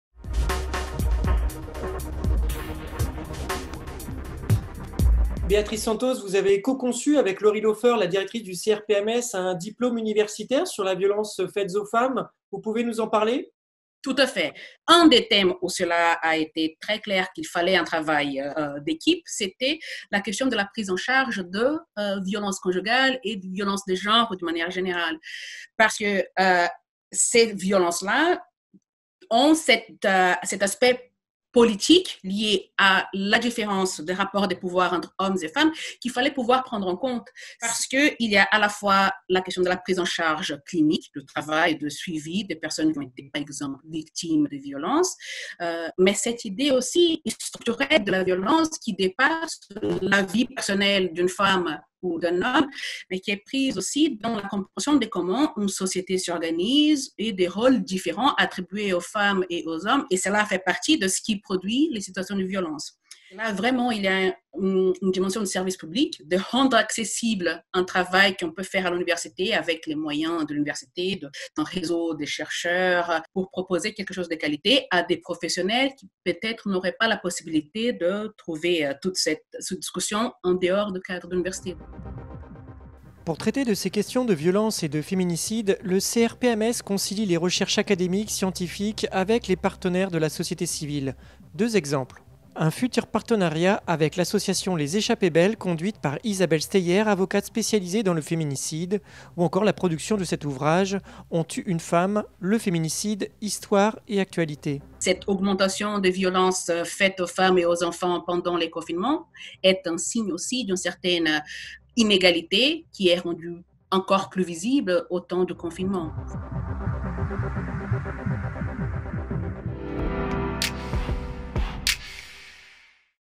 Une formation sur la question des violences faites aux femmes
Un diplôme universitaire (DU) est proposé par Université de Paris pour comprendre et se former sur la thématique des violences faites aux femmes et au genre. Découvrez cette formation à travers cet entretien avec Béatriz Santos.